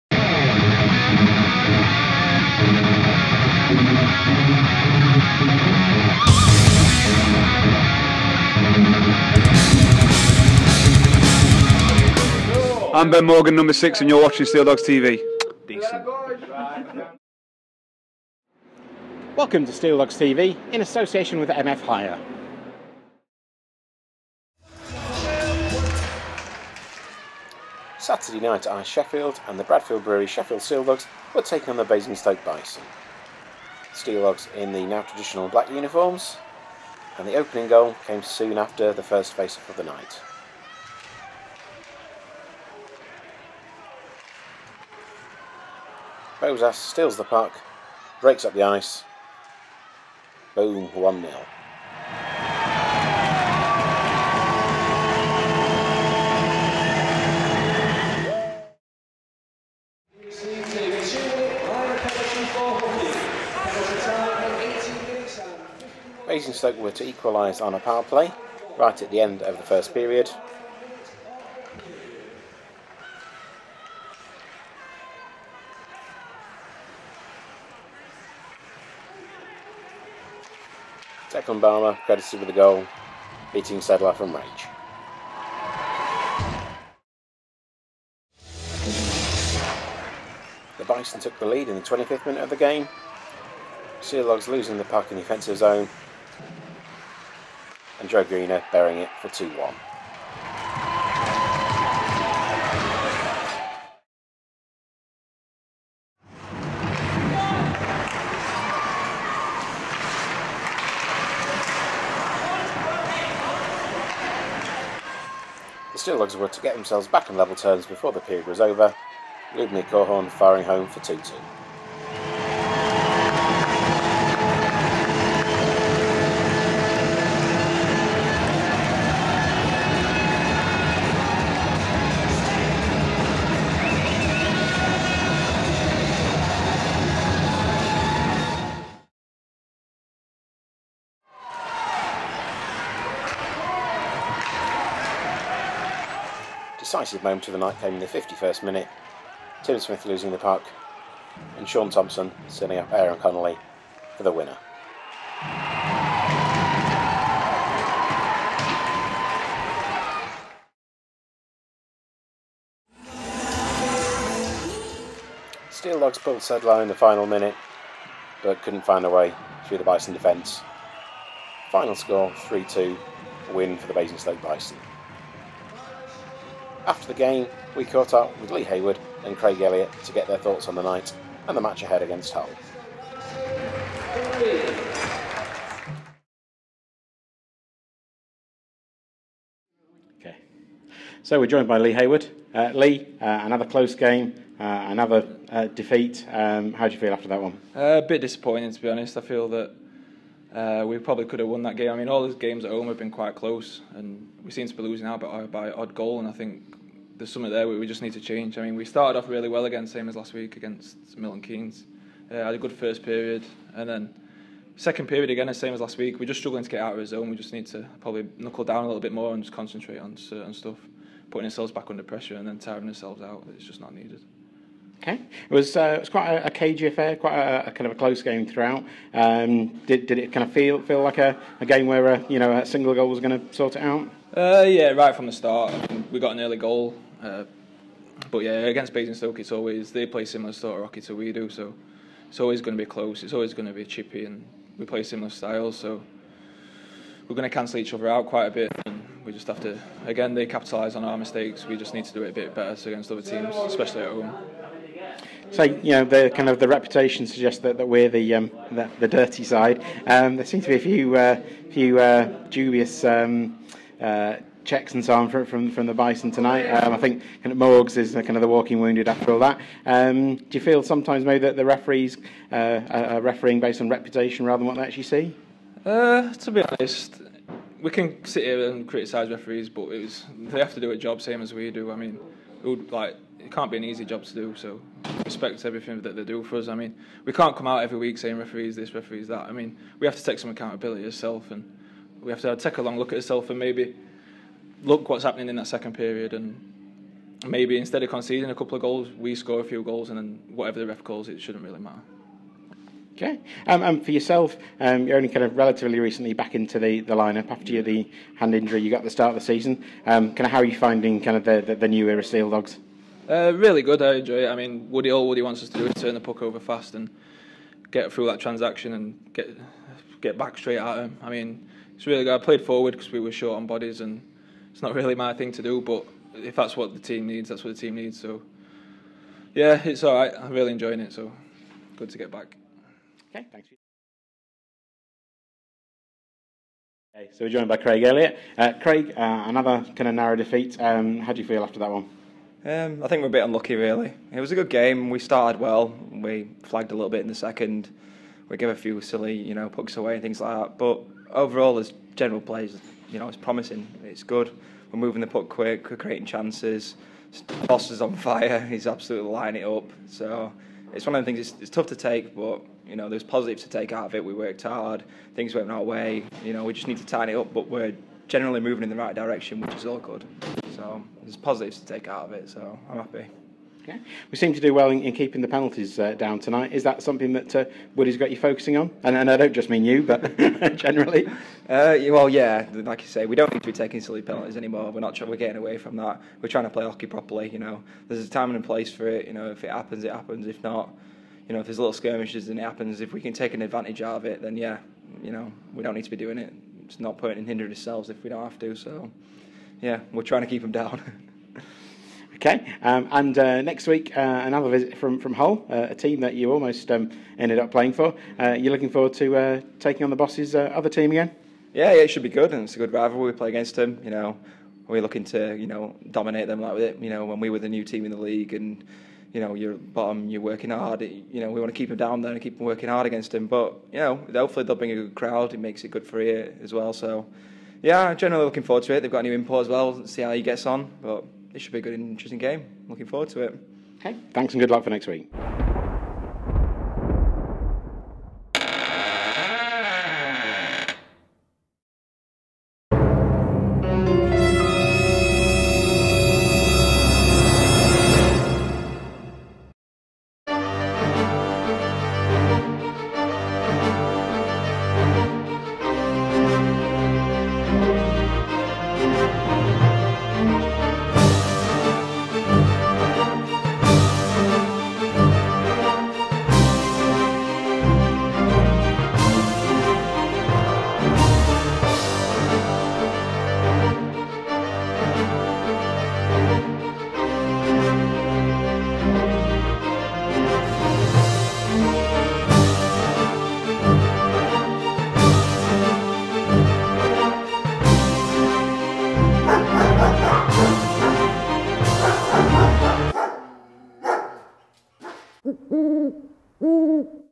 SteeldogsTV - Episode 7: Sheffield Steeldogs vs Basingstoke Bison
This week we feature the visit of the Basingstoke Bison with match highlights and interviews with Lee Haywood and Craig Elliott.

And don't forget Saturday night's big halloween clash with the Hull Pirates!